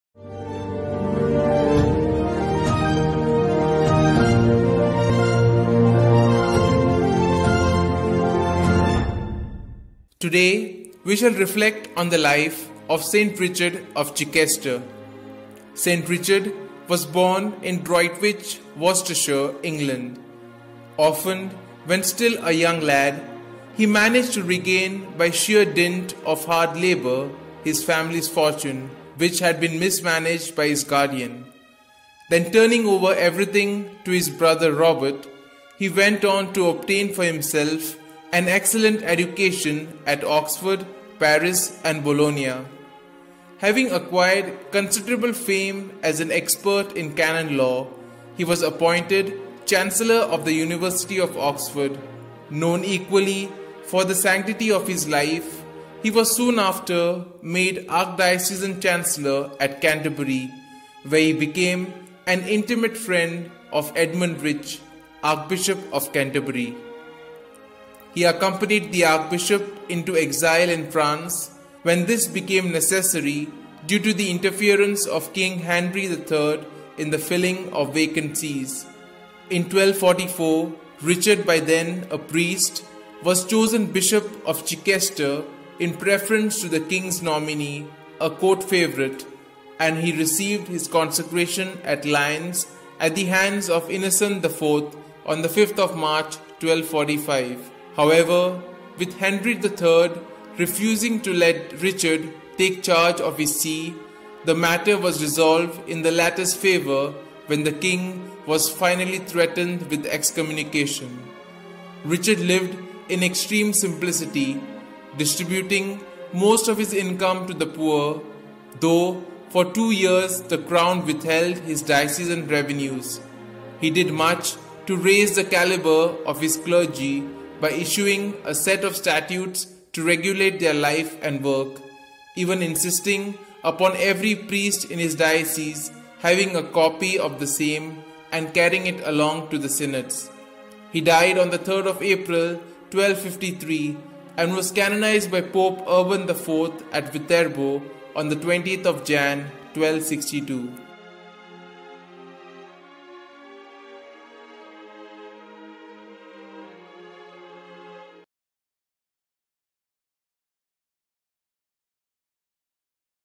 St Richard Of Chichester - Saint of the Day with Fr Lindsay - 3 April 2022
Here is a brief reflection on the life of St Richard Of Chichester presented by Fr Lindsay Lobo.